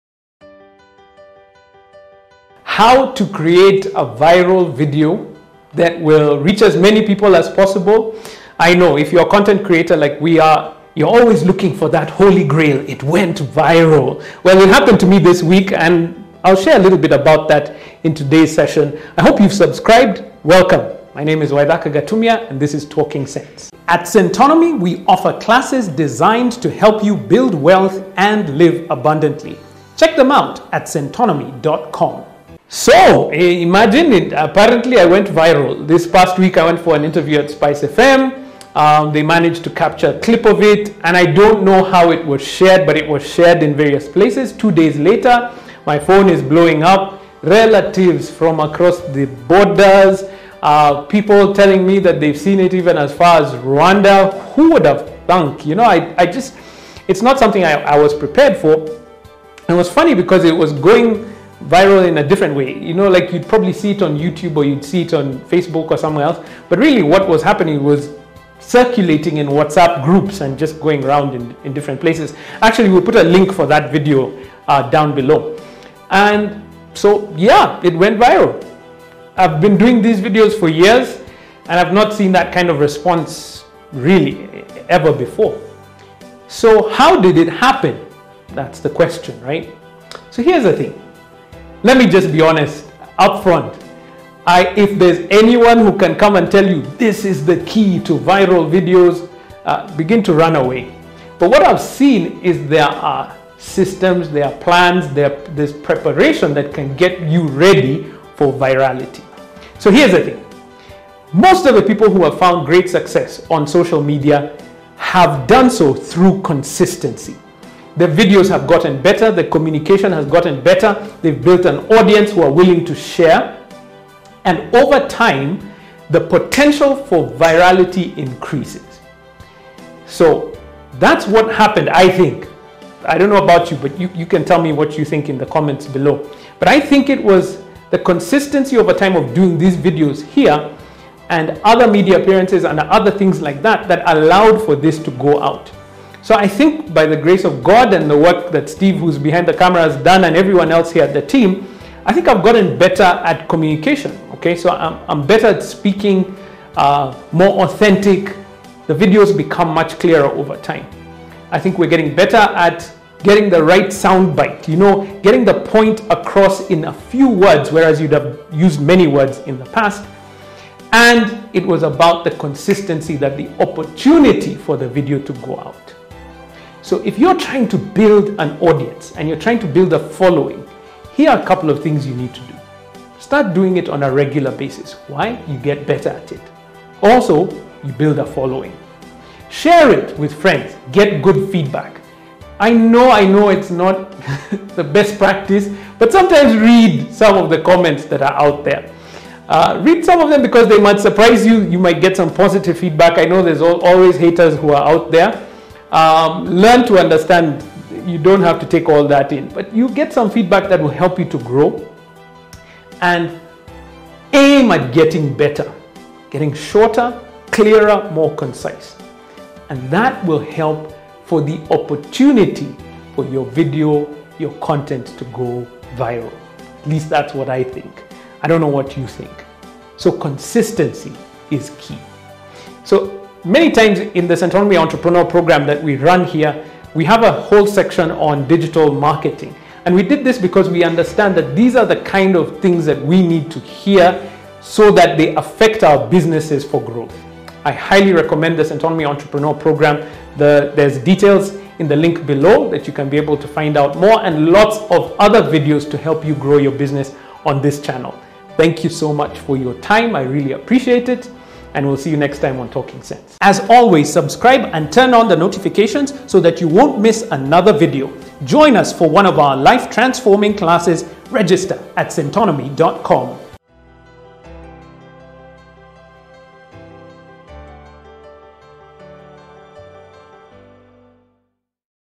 Let's Go Viral ~ Waithaka Gatumia
I was from a radio interview talking about retirement, later that video went viral. We didn't sponsor or push it out in any way but it got momentum on it's own. so here are my thoughts of how you as a business person can also make your business be known.

Get Social With Us:
CENTONOMY ENTREPRENEUR: